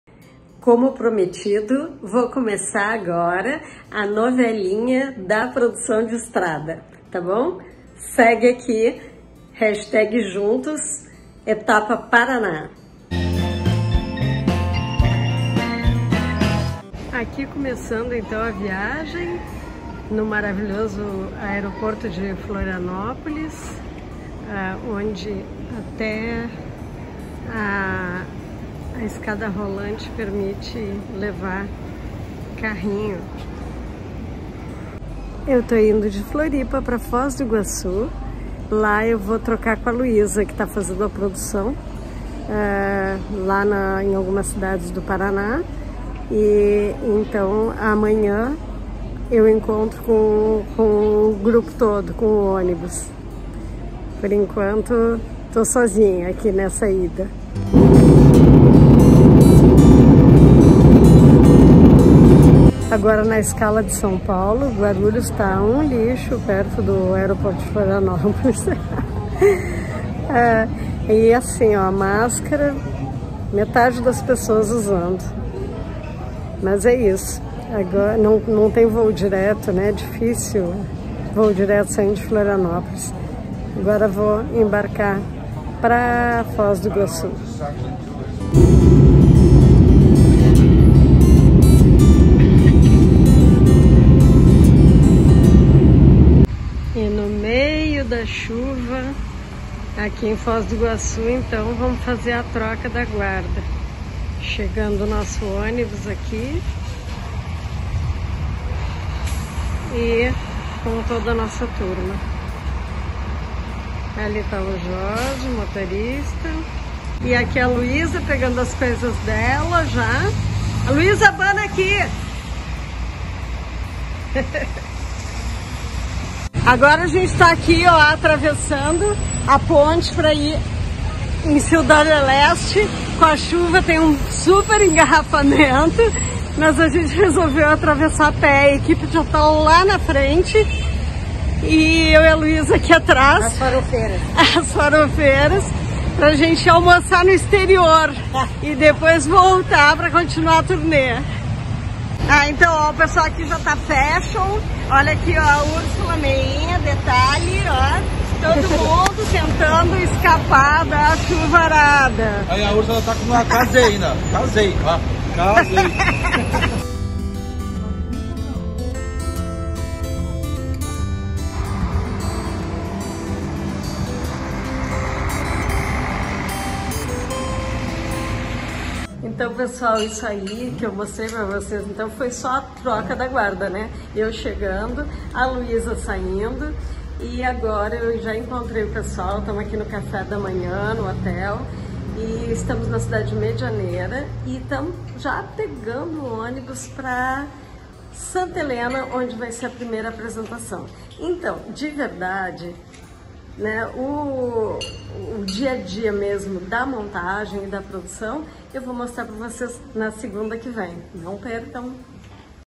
Juntos na Estrada - Parte 1
Vem com a gente para o oeste do Paraná, acompanhar de perto o trabalho de um produtor de estrada.  No caso, eu! Na primeira parte é só a viagem para encontrar o grupo e trocar com a Luiza, que estava antes. Bora?